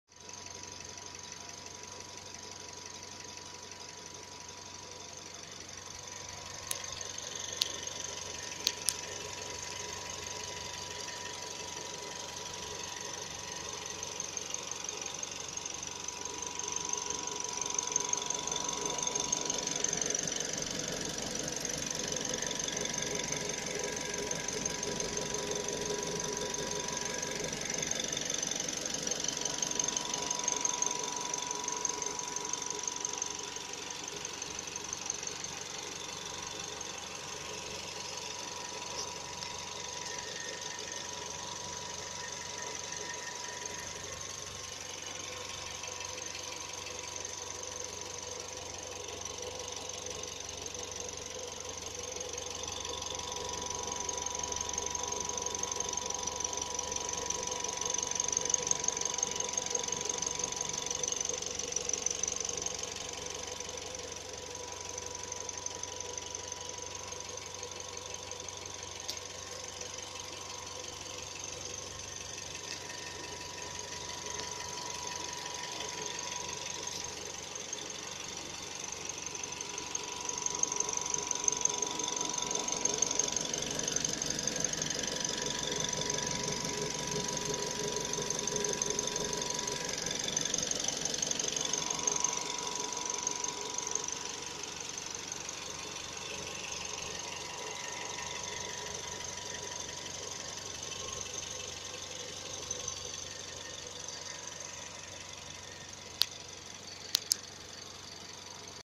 Работа двигателя Сузуки SX4. Working Suzuki SX4 engine
2008 г.в., 46 т.км пробег. Ваше мнение специалисты?
2008 of release, run of 46 thousand kilometers. Your opinion experts?